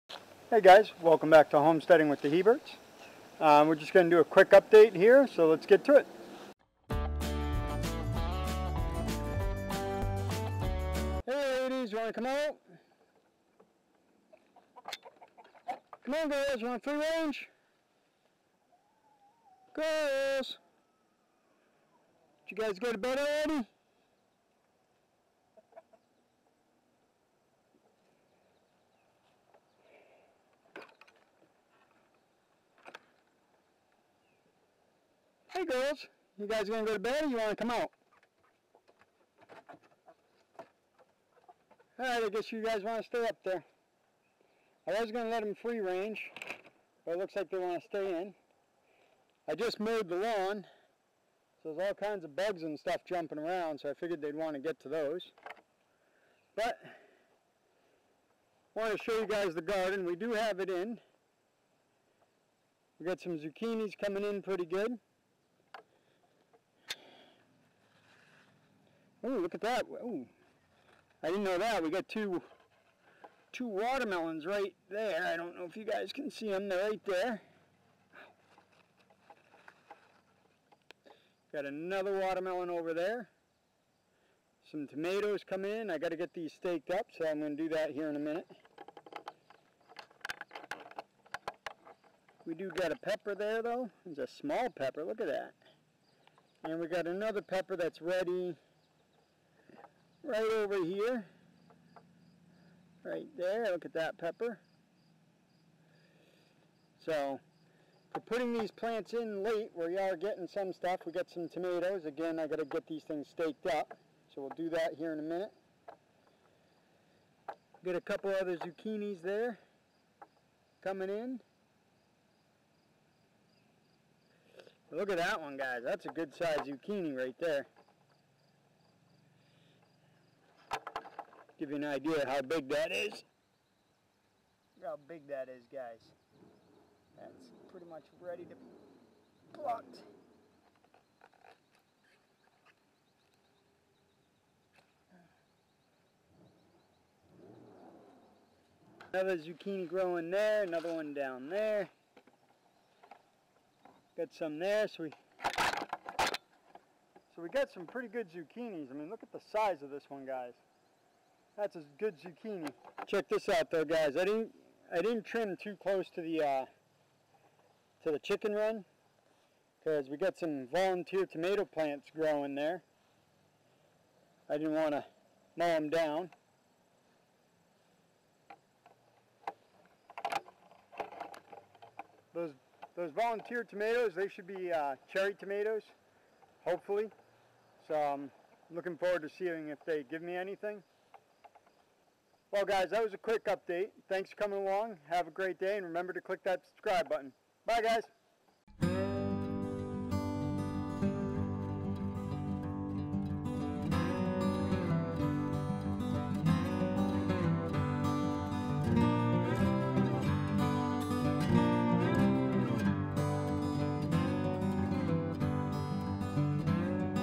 LOOK AT THE SIZE OF THIS ZUCCHINI.
LOOK AT THE SIZE OF THIS ZUCCHINI. In this garden update we look at the chickens, and the garden. This garden was put in the ground early July. we can now start to see some fruit for our labor.